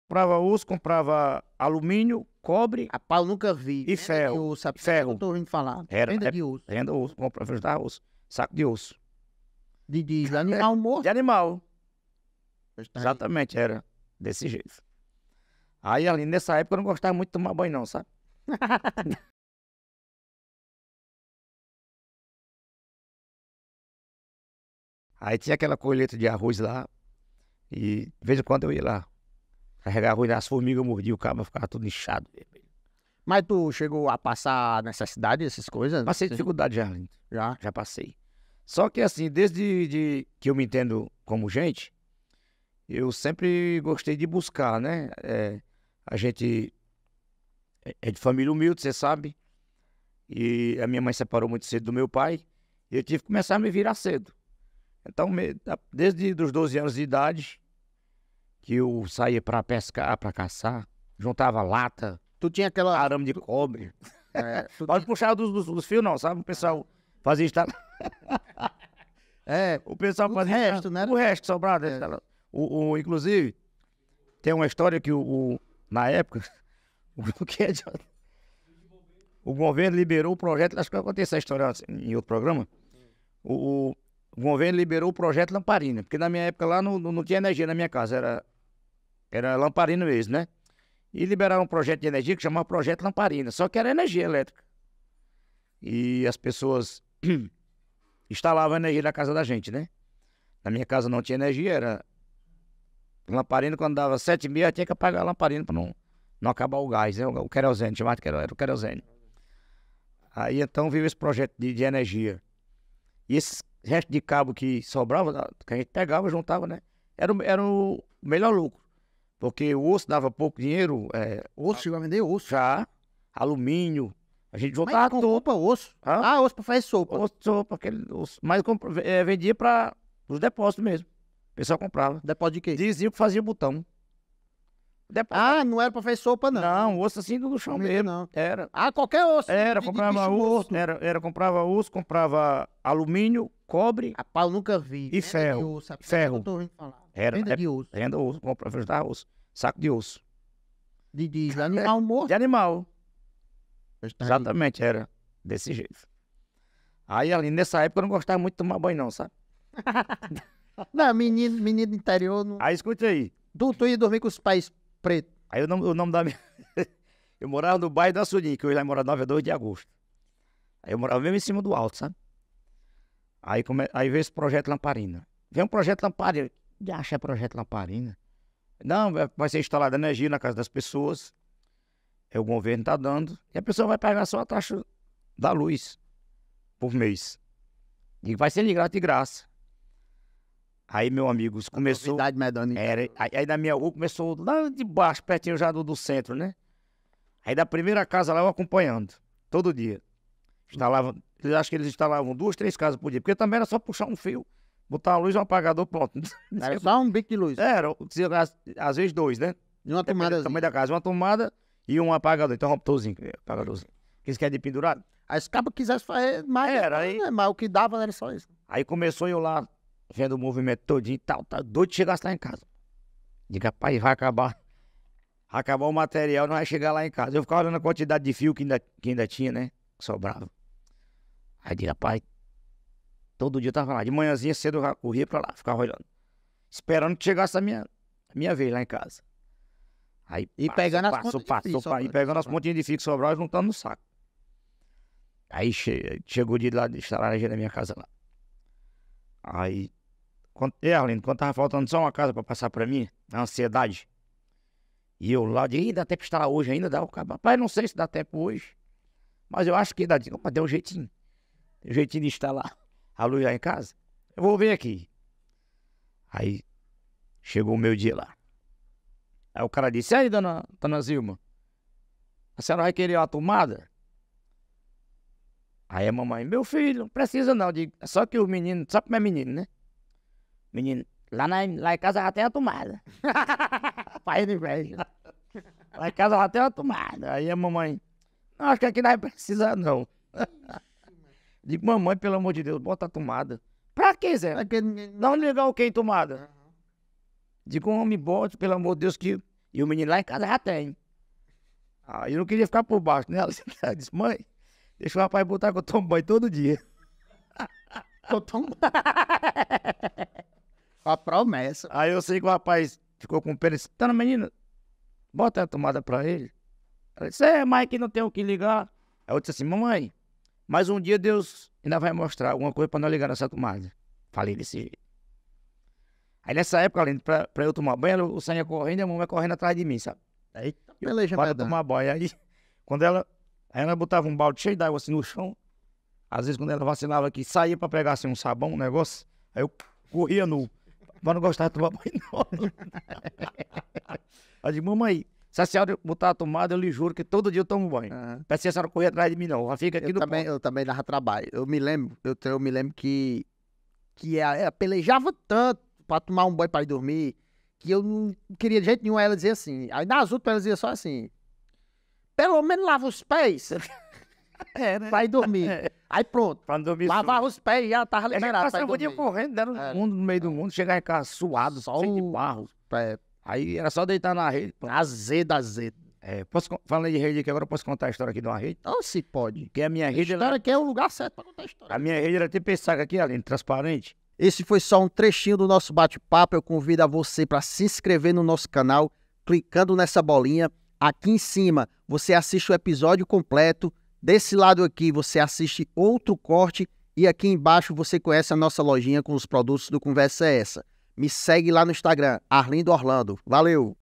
Zé Cantor juntava ossos de animais pra vender na infância em Morada Nova.
Zé Cantor conta como era sua infância no interior do Ceará na cidade de Morada Nova, chegou a juntar ossos pra vender.

REALIZAÇÃO: Fortal Filmes
ANFITRIÃO: Arlindo Orlando
PRODUÇÃO: Tamara Nunes